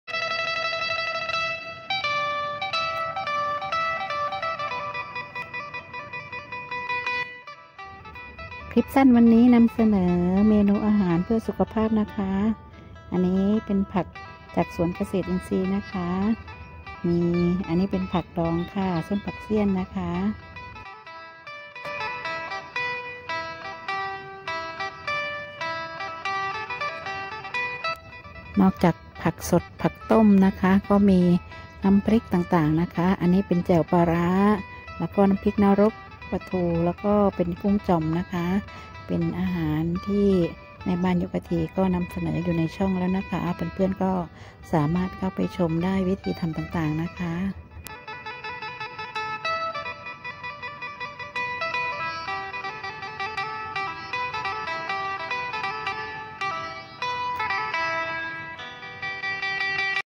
เมนู​อาหาร​เพื่อ​สุขภาพ​เมนูคลีน...Shorts Clip คลิปสั้นๆ
วันนี้แม่บ้านยุคไอทีมีคลิปสั้นๆ  ทักทายเพื่อนด้วยเมนูอาหารเพื่อสุขภาพ  มาให้ชมเรียกน้ำย่อยกันนะคะ